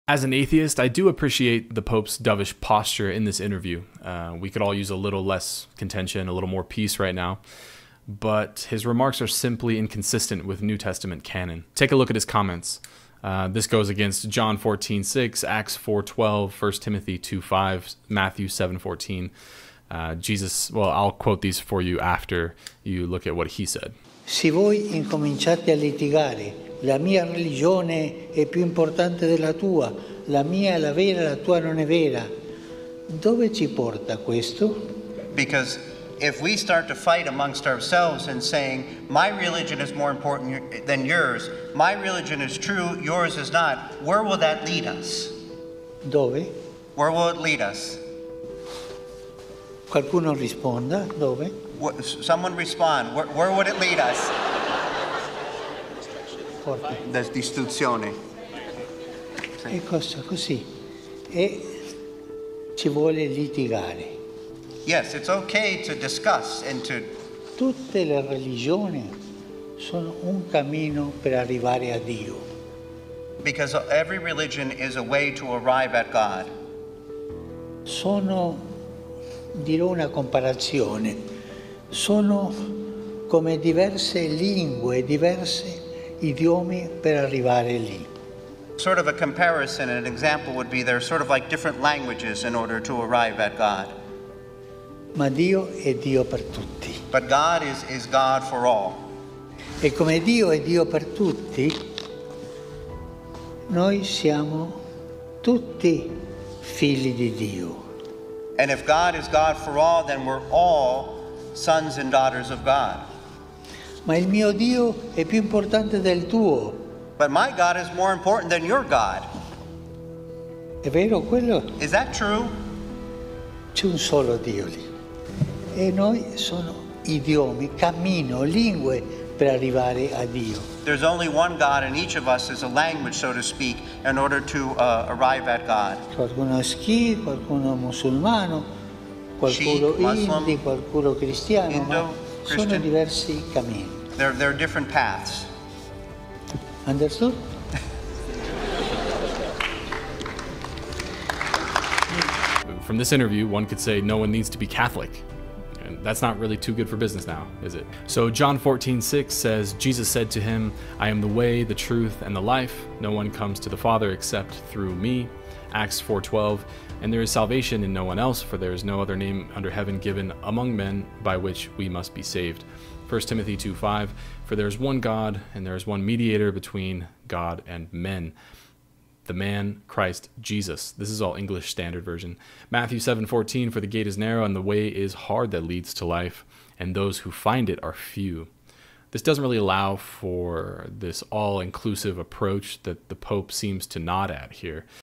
Pope: Jesus is not the only way to God. All religions are paths to God.
Recent comments from Pope Francis shocked the Catholic world. Is Catholicism even necessary? Is Jesus the only way to heaven?